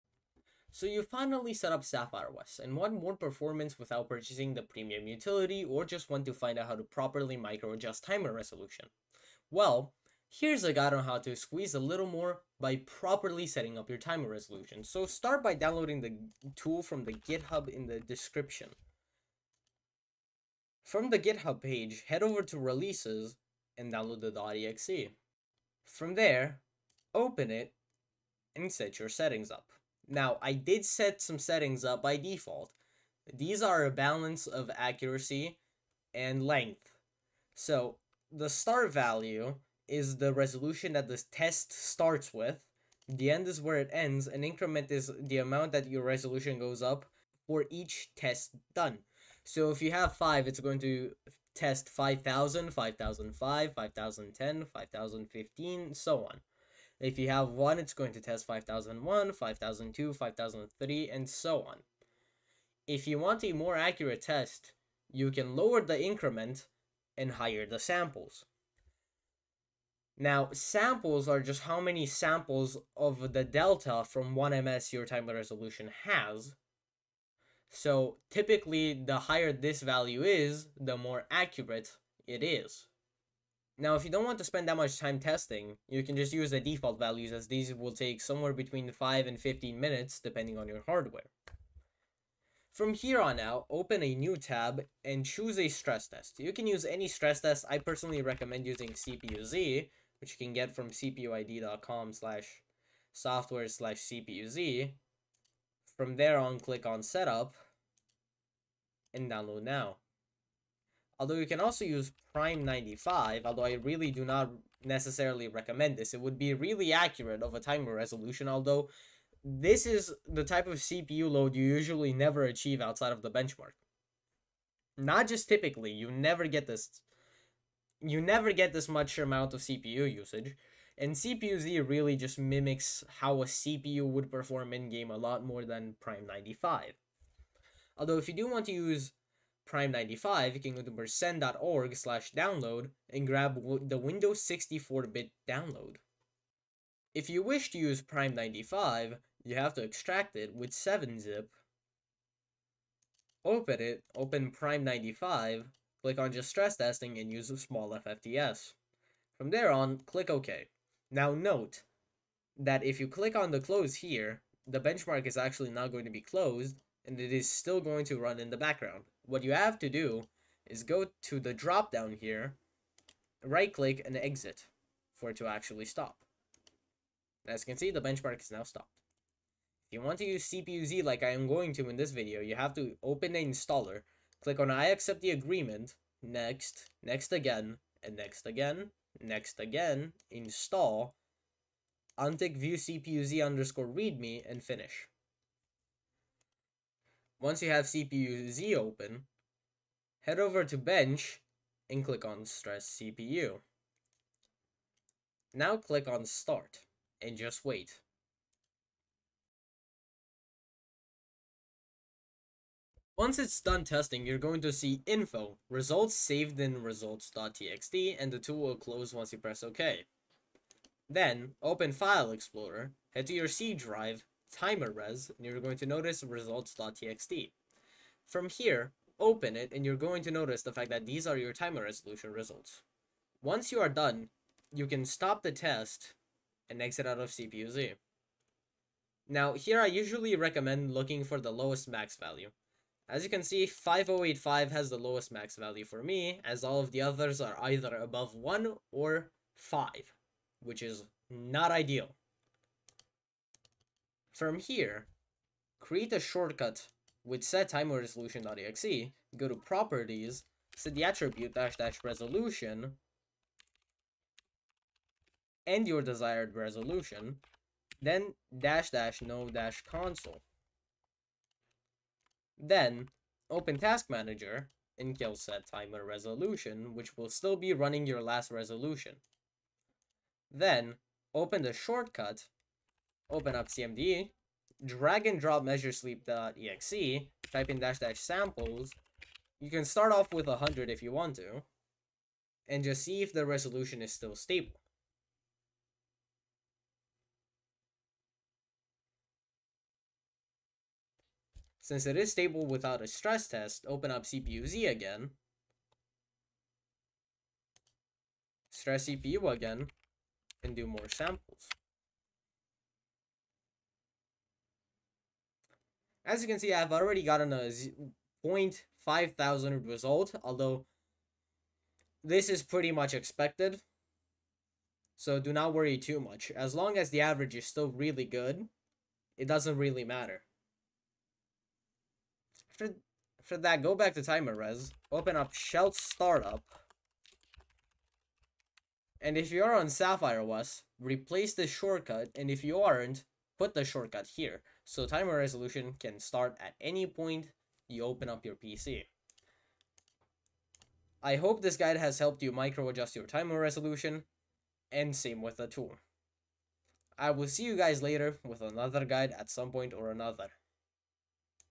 How To Properly Set up Timer Resolution on Windows
Timer resolution is the rate at which the system's hardware clock generates interrupts that allow the scheduler to perform tasks such as timekeeping
on most systems the minimum is 64hz a.k.a 15.625ms A lower frequency means less cpu overhead and fewer interrupts but bad precision whilst a higher frequency like the maximum of 2khz means a interrupt is generated at a rate of 0.5ms potentially ensuring a higher precision timer and multitasking responsiveness at the cost of some overhead and in this video I am showcasing how to properly set it up